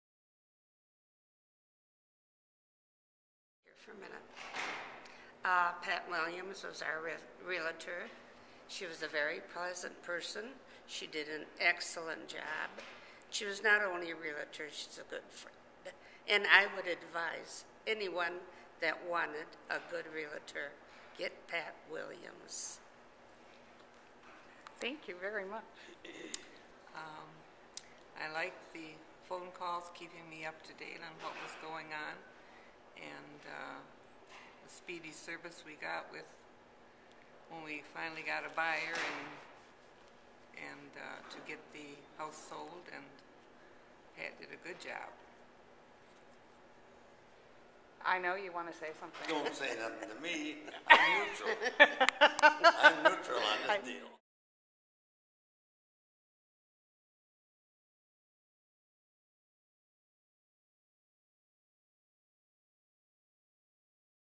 Testimonial for Farmington Hills Real Estate Sale
Sellers state what they think about Pat Williams (Realtor with Century 21) real estate services.